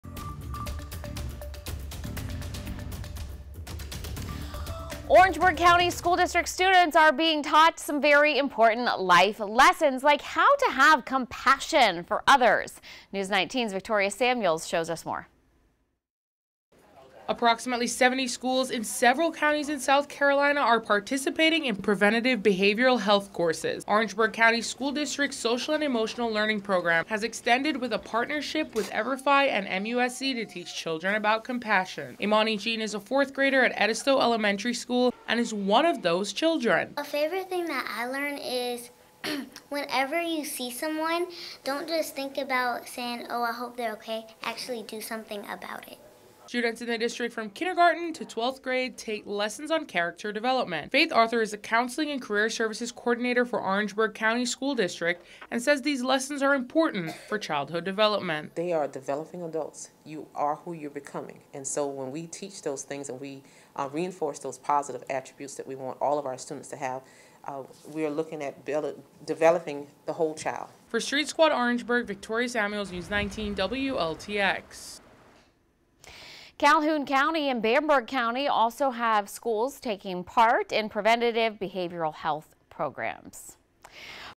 Orangeburg County students learning important life lessons, like compassion
Orangeburg County School District's social and emotional learning program has extended a partnership with EVERFI and MUSC to teach children about compassion.